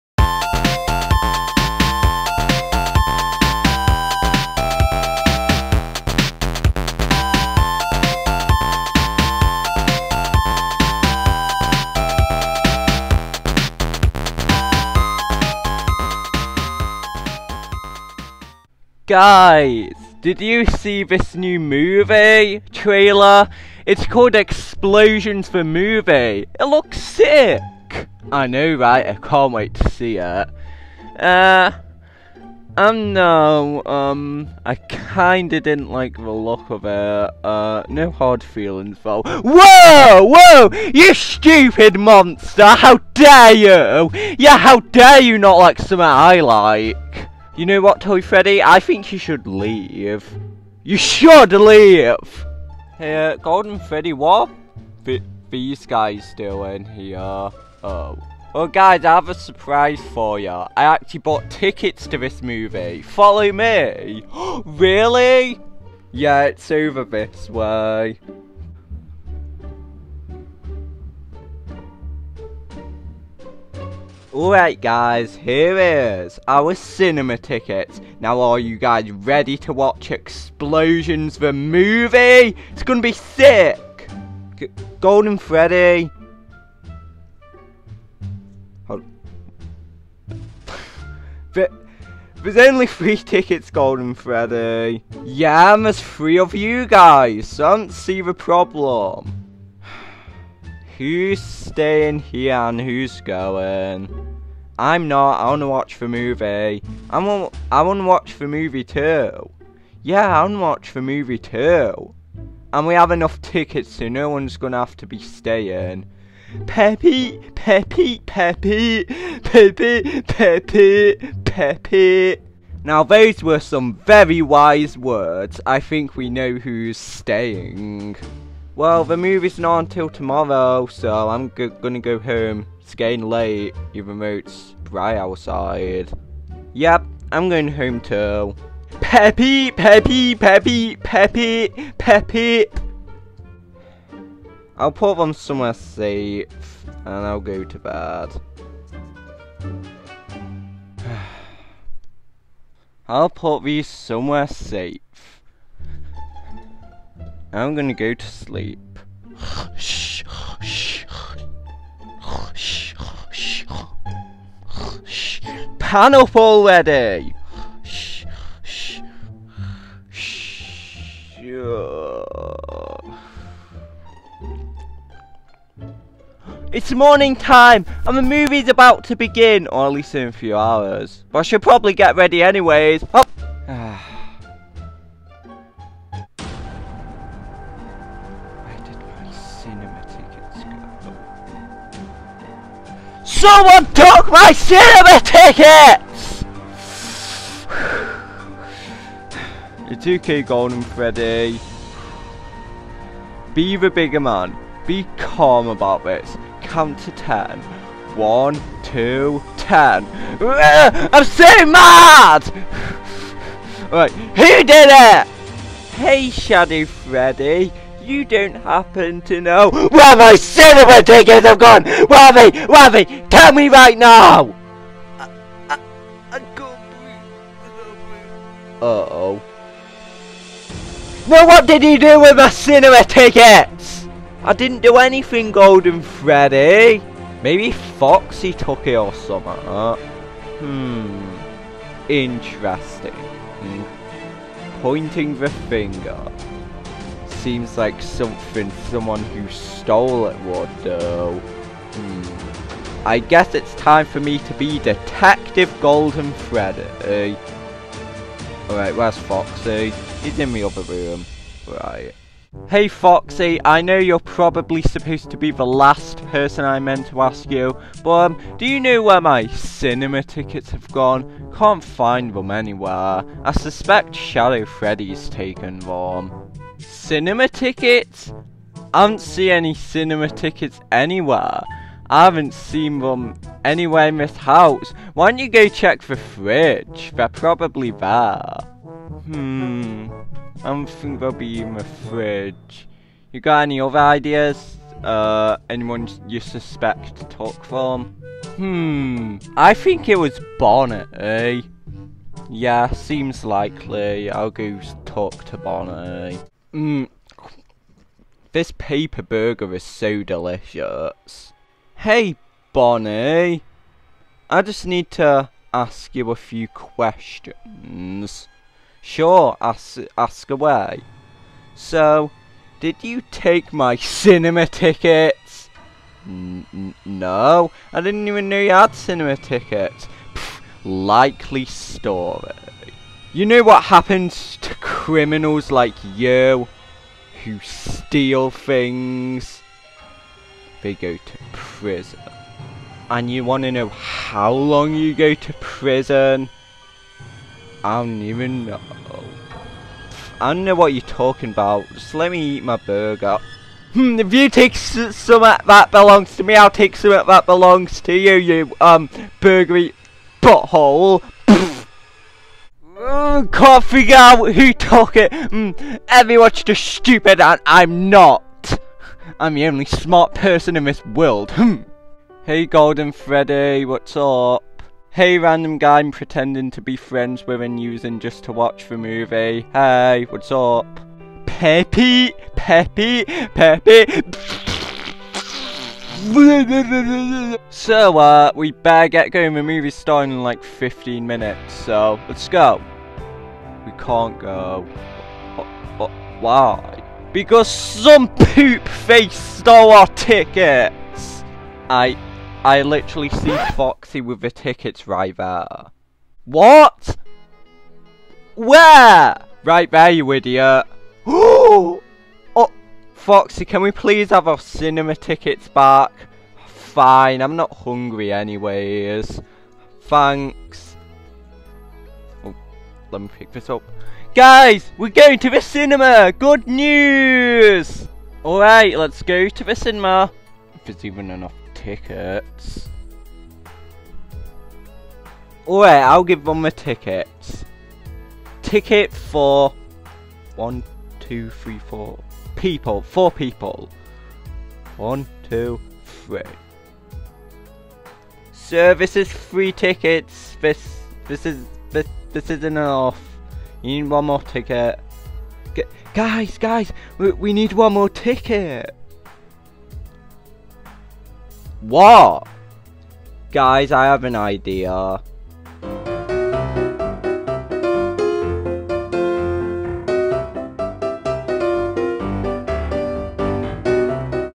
Bonnie And Foxy: Golden Freddy’s Tickets
Golden Freddy and his friends want to watch the new movie, but the tickets go missing…